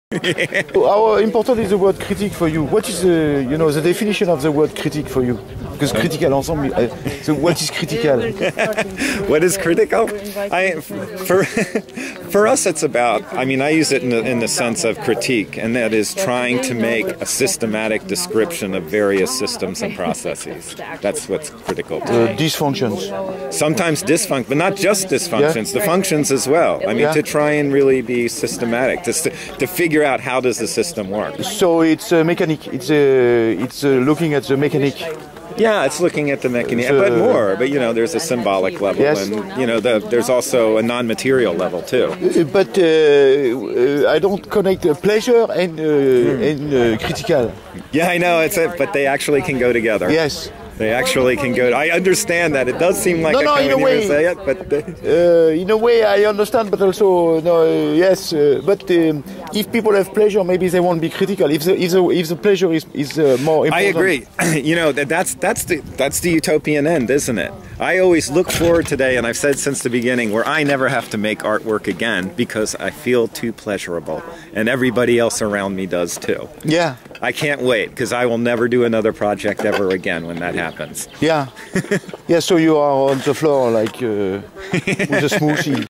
Critical Art Ensemble (CAE) / what is the definition of the word "CRITIC for you ?Steve Kurtz
format artist Thierry Geoffroy / Colonel is very interest in words , especially the word "Critic  " . He had the chance to meet  Steve Kurtz at Documenta Kassel and ask him some questions and  even proposed him  "a  critical run " ,that unfortunatly  did not happend  It was in fact a long time Colonel wanted to have the chance  to exchange with Critical Art Ensemble .More to come in Kassel in the CAE space ...